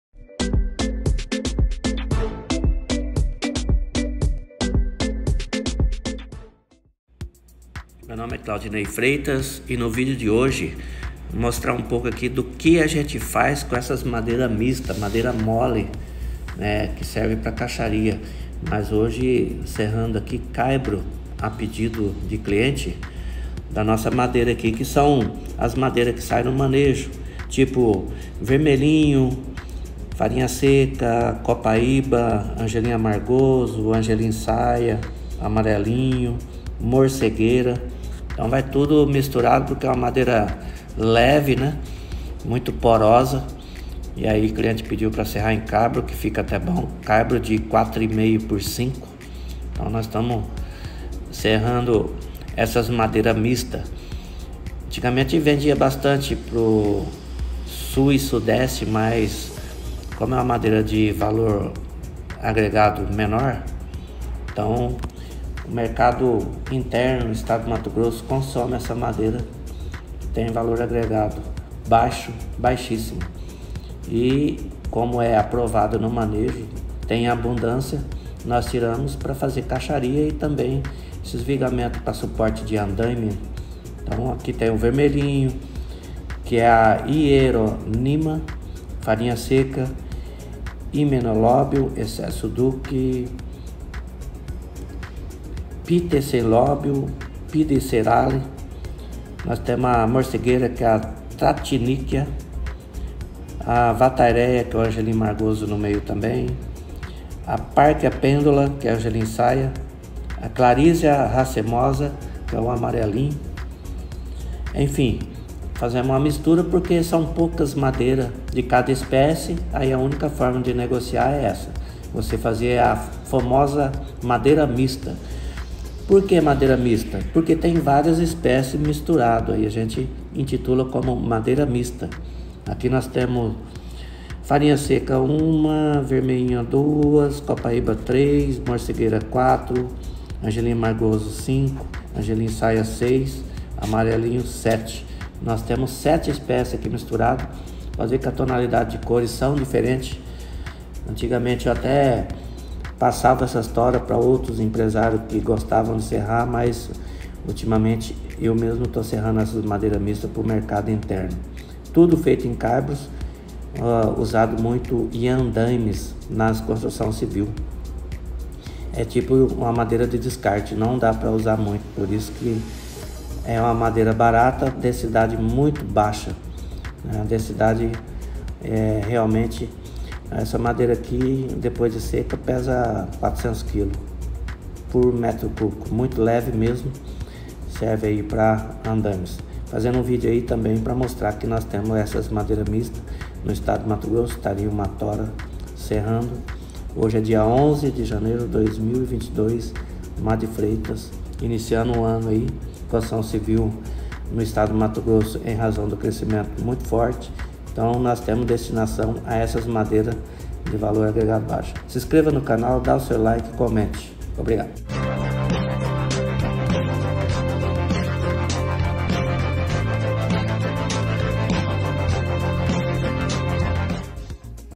Madeiras Mixta de Baixa Densidades, Morcegueira, Vermelhinho, Copaíba, Angelim Saia, Amarelinho.
Madeira que pesam 520 kilos por m/3 serve para cacharias e Andaimes nas obras, consumido mercado interno Mato Grosso, Características sensoriais: cerne e alburno pouco distintos pela cor, cerne bege-rosado ou bege-amarelado; cheiro e gosto imperceptíveis; densidade baixa; moderadamente dura; grã direita ou irregular; textura média; superfície irregularmente lustrosa; camadas de crescimento pouco distintas, delimitadas por zonas fibrosas ligeiramente mais escuras.

Descrição anatômica macroscópica:
• Parênquima axial: invisível mesmo sob lente.
• Raios: visíveis apenas sob lente no topo e na face tangencial, finos e numerosos.
• Vasos: visíveis a olho nu; pequenos a médios; poucos; solitários e múltiplos; vazios ou obstruídos por tilos.
• Camadas de crescimento: pouco distintas, demarcadas por zonas fibrosas mais escuras.
• Máculas medulares: presentes em alguns espécimes.

Fonte: (IPT,1983)

DURABILIDADE / TRATAMENTO

Durabilidade natural: não durável aos fungos apodrecedores. (SUDAM/IPT,1981)

Tratabilidade: cerne de difícil preservação com penetração parcial e periférica em processo sob pressão (retenção de preservativo oleossolúvel abaixo de 100 kg/m3). Alburno moderadamente fácil de preservar, com penetração total e uniforme de preservativos em processo sob pressão (retenção de preservativo oleossolúvel entre 200 kg/m3 e 300 kg/m3). (IBDF,1981)

CARACTERÍSTICAS DE PROCESSAMENTO

Trabalhabilidade: a Madeira de amesclão é fácil de serrar, moderadamente fácil de aplainar apresentando superfícies radiais ásperas. (IBAMA,1997a) Recebe acabamento de regular a excelente.  (INPA,1997)

Secagem: a secagem em estufa é rápida, apresentando tendência a rachaduras moderadas a fortes; encanoamento e torcimento moderados.

Programa de secagem é sugerido por (IBAMA,1997a)

PROPRIEDADES FÍSICAS

Densidade de massa (r):
• Aparente a 12% de umidade (rap, 12): 520 kg/m³
• Madeira verde (rverde): 955 kg/m³
• Básica (rbásica): 440 kg/m³

Fonte: (IBAMA,1997a)

Contração:
• Radial: 5,1 %
• Tangencial: 7,2 %
• Volumétrica: 11,8 %

Resultados obtidos de acordo com a Norma COPANT.
Fonte: (IBAMA,1997a)
Para comparar esses valores de contração (CCOPANT) com aqueles obtidos pela Norma ABNT (CABNT) é necessário transformá-los usando a equação: CABNT = CCOPANT / (1 - CCOPANT / 100).

PROPRIEDADES MECÂNICAS

Flexão:
• Resistência (fM):
      Madeira verde: 49,7 MPa
      Madeira a 12% de umidade: 76,3 MPa
• Módulo de elasticidade - Madeira verde: 7649 MPa
• Módulo de elasticidade - Madeira a 12%: 9611 MPa

Fonte: (IBAMA,1997a)

Compressão paralela às fibras:
• Resistência (fc0):
      Madeira verde: 24,8 MPa
      Madeira a 12% de umidade: 44,1 MPa

Fonte: (IBAMA,1997a)

Outras propriedades:
• Cisalhamento - Madeira verde: 6,6 MPa
• Cisalhamento - Madeira a 12%: 8,2 MPa
• Dureza janka paralela - Madeira verde: 3501 N
• Dureza janka paralela - Madeira a 12%: 4609 N
• Dureza janka transversal - Madeira verde: 2520 N
• Dureza janka transversal - Madeira a 12%: 3099 N
• Tração normal às fibras - Madeira verde: 3,1 MPa
• Tração normal às fibras - Madeira a 12%: 3,5 MPa

Fonte: (IBAMA,1997a)

USOS

Construção civil:
• Leve interna, utilidade geral:
      cordões
      guarnições
      rodapés
      forros
      lambris

Mobiliário:
• Utilidade geral:
      móveis estândar
      partes internas de móveis inclusive daqueles decorativos

Outros usos:
      chapas compensadas
      lâminas decorativas
      artigos de esporte e brinquedos
      embalagens